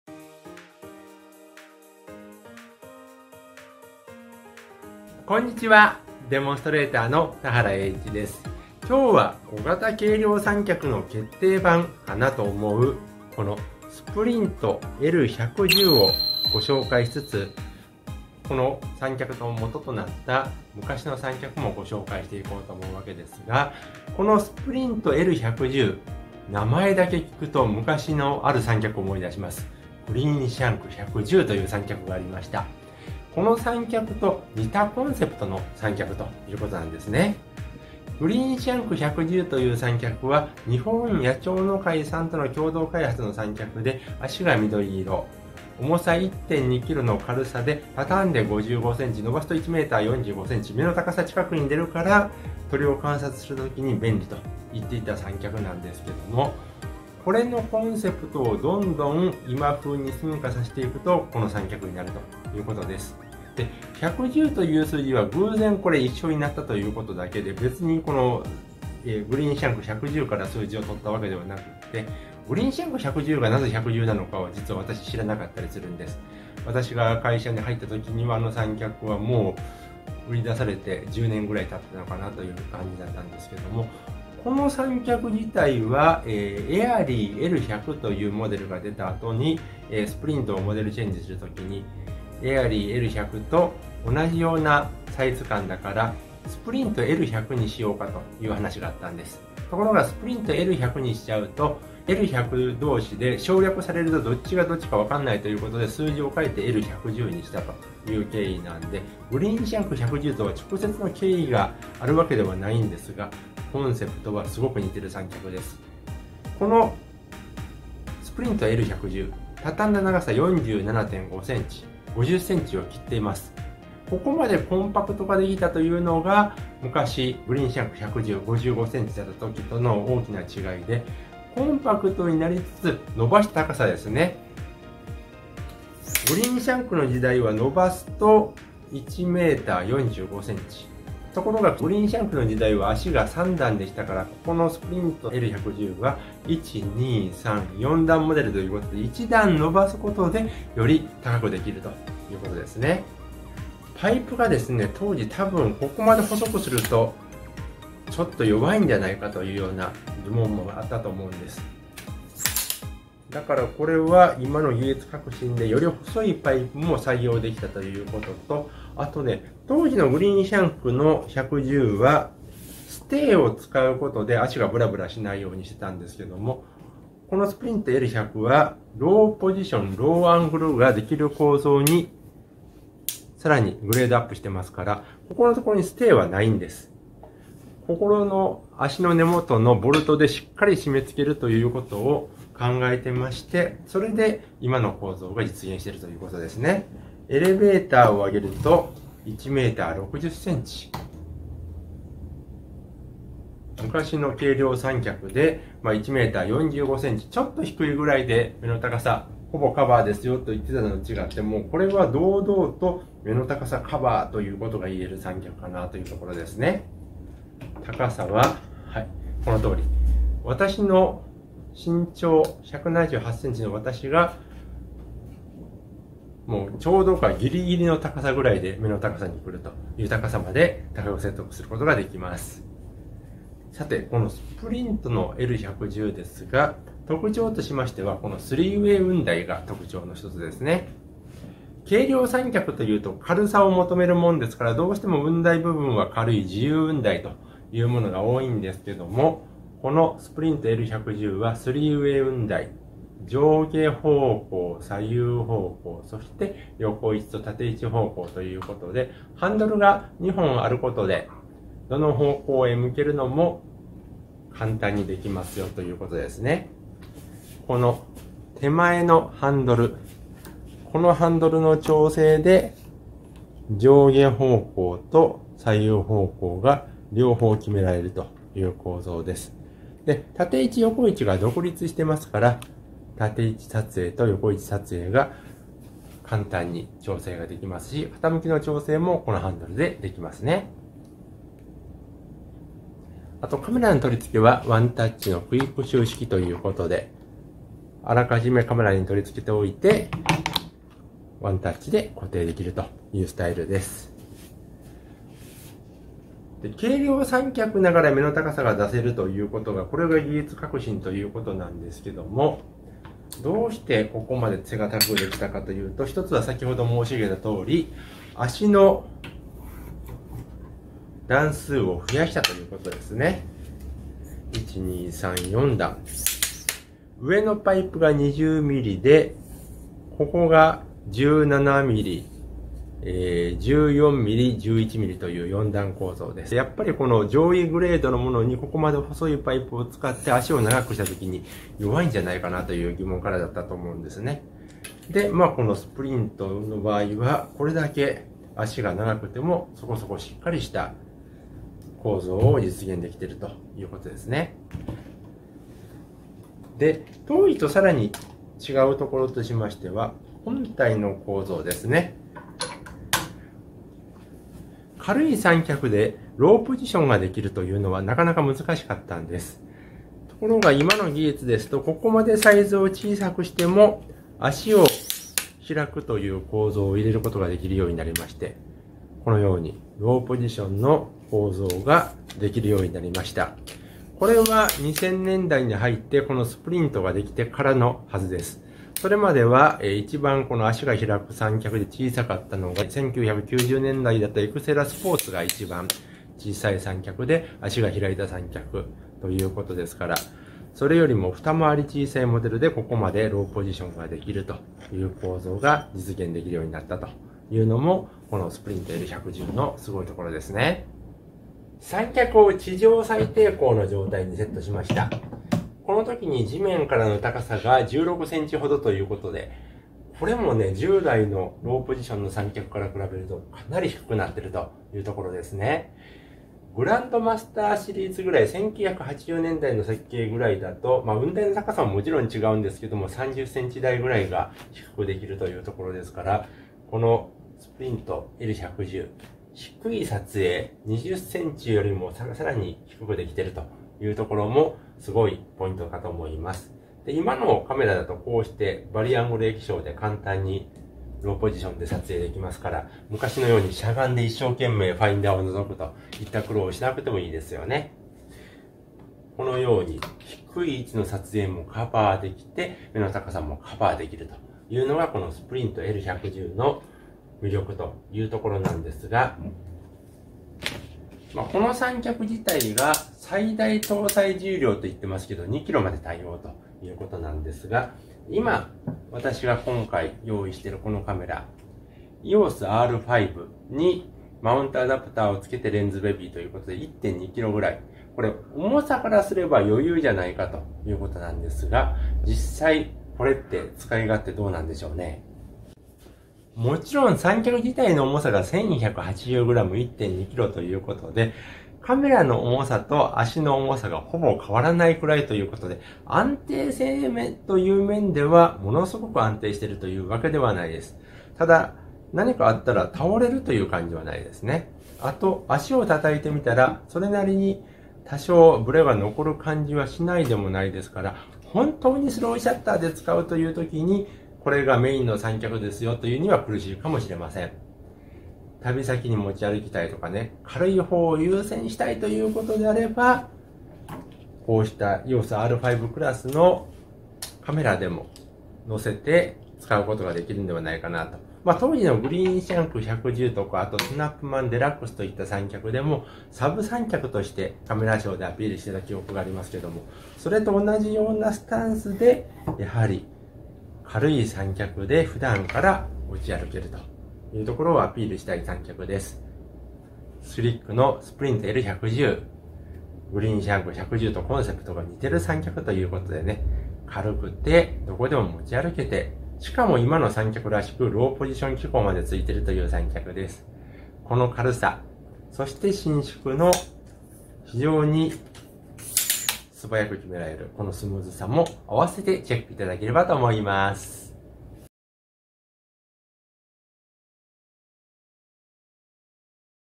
【110という軽量三脚！】スプリントL110は背が高くて軽い！（動画No.1115）
スプリントL110は、1,180gの軽さながら、160cm越えの高さを確保できる三脚です。
昔、似たコンセプト（軽くて背が高い）の三脚に「グリーンシャンク110」というのがありましたが、関係は？？？